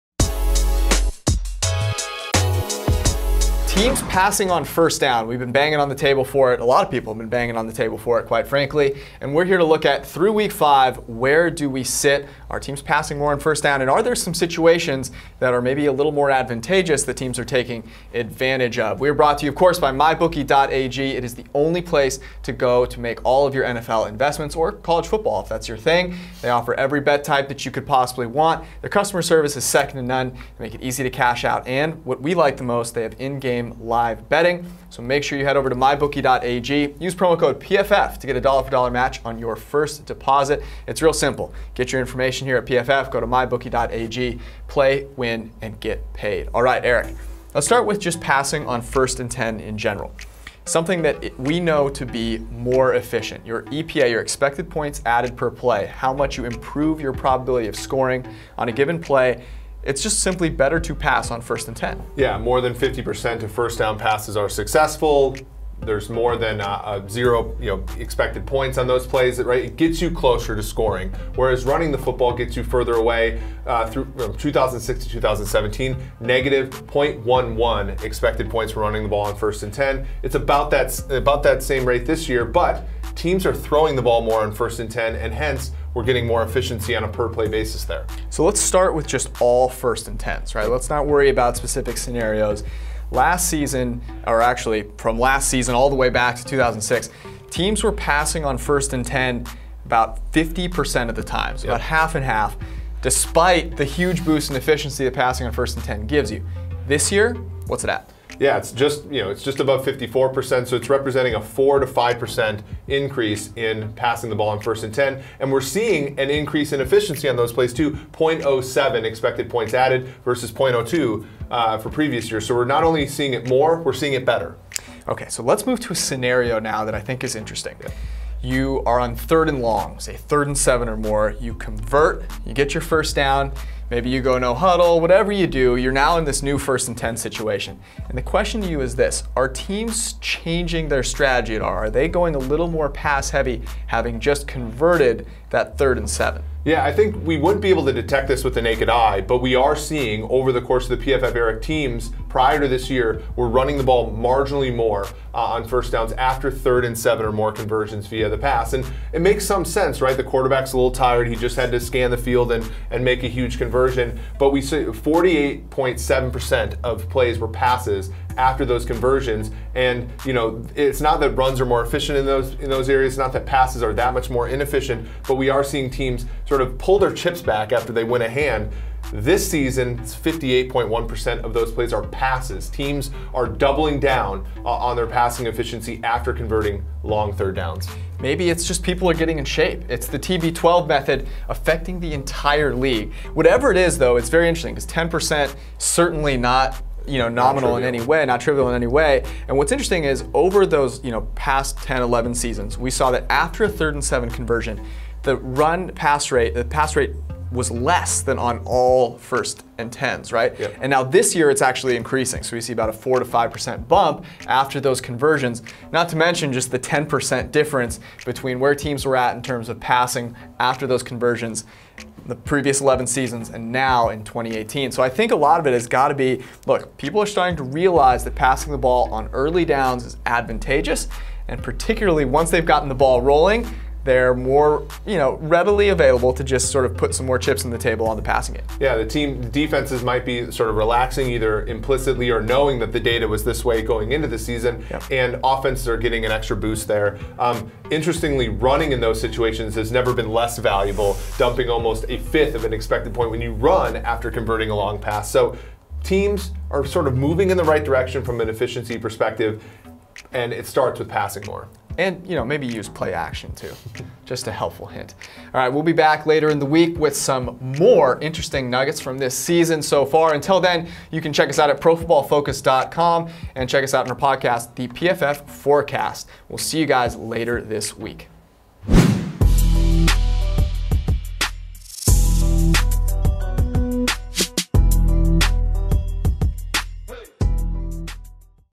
NFL Trends: Pass vs. Run on 1st Down | PFF
PFF data scientists George Chahrouri and Eric Eager discuss the EPA (expected points added) when running on 1st down compared to passing on first down.